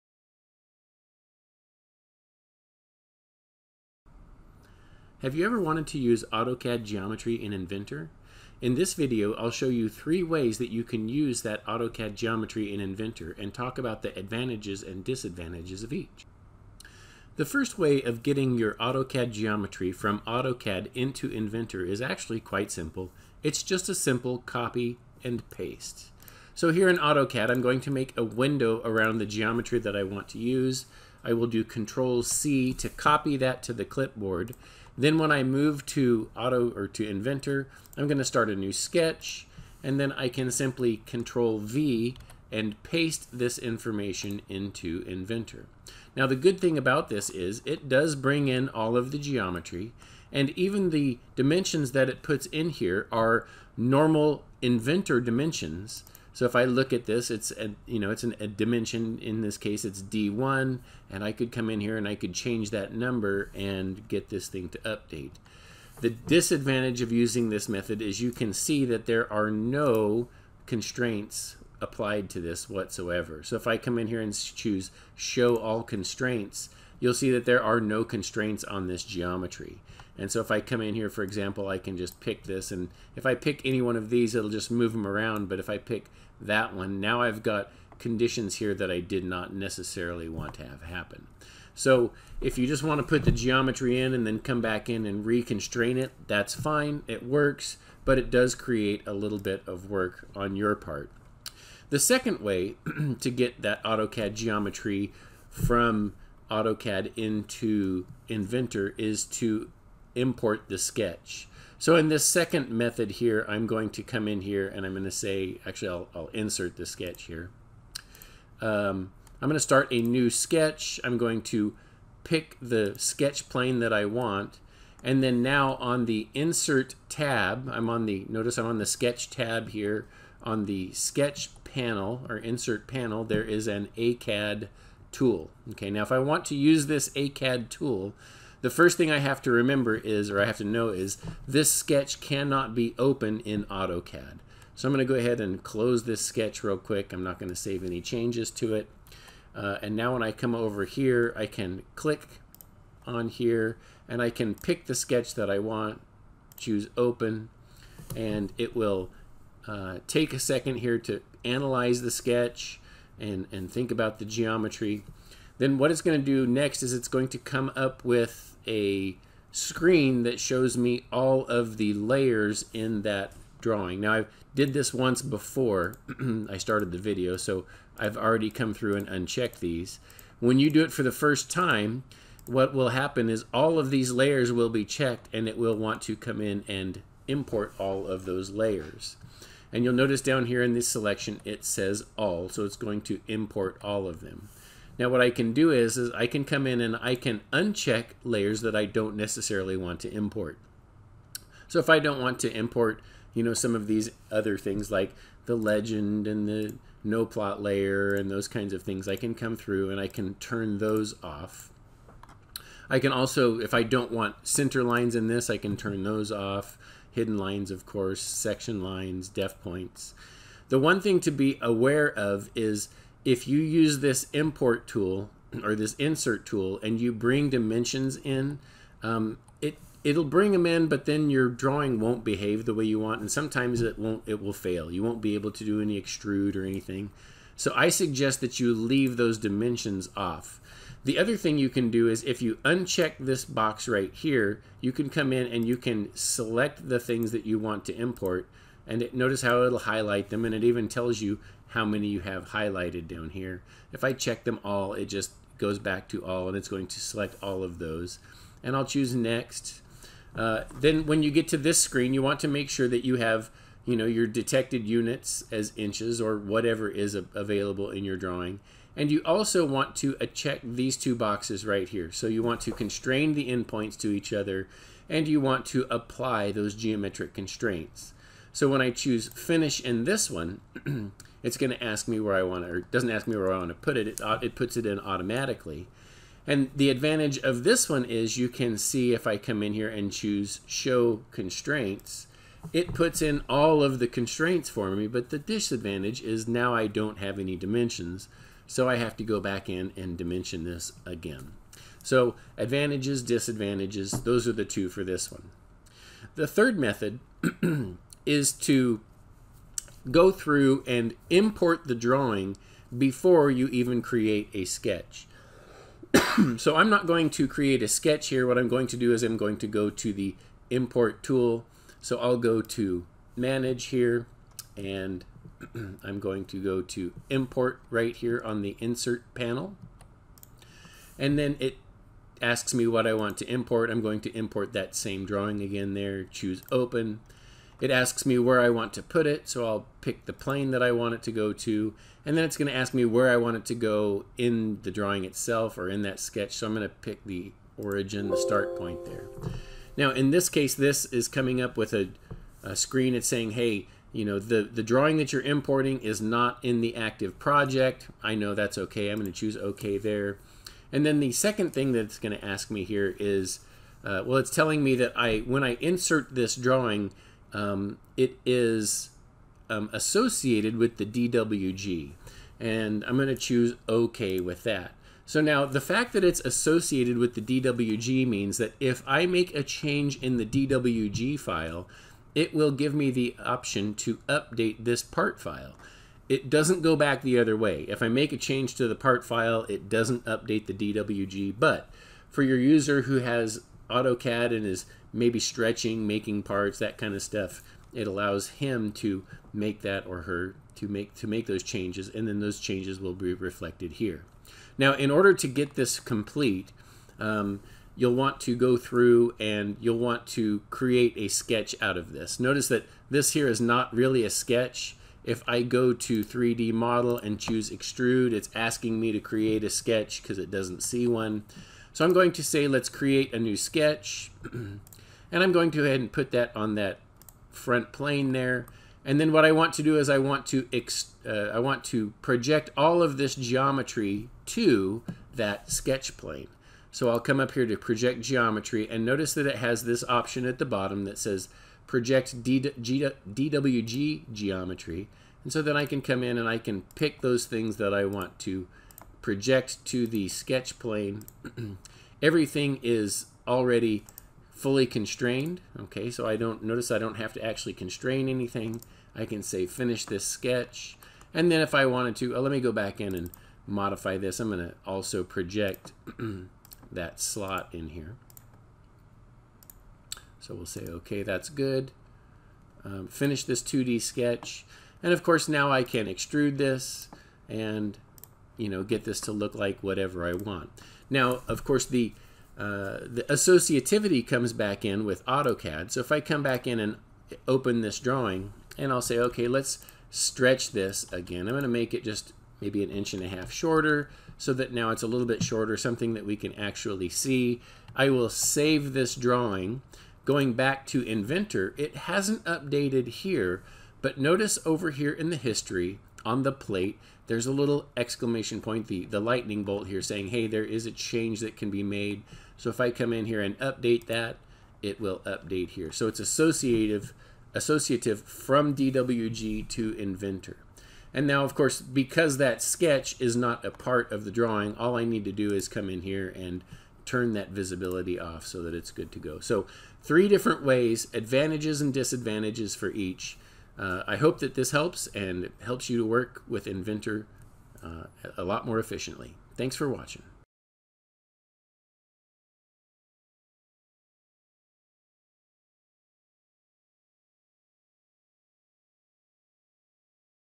Using DWG Geometry In Inventor
Thanks to @anotherwhitezombie for the tip in a previous version of this video!
There are three ways of bringing drawing geometry from AutoCAD to Inventor. In this video, we will look at the three methods and look at the advantages of each. This channel is dedicated to practical solutions for your problems with AutoCAD, AutoCAD Electrical, and Autodesk Inventor.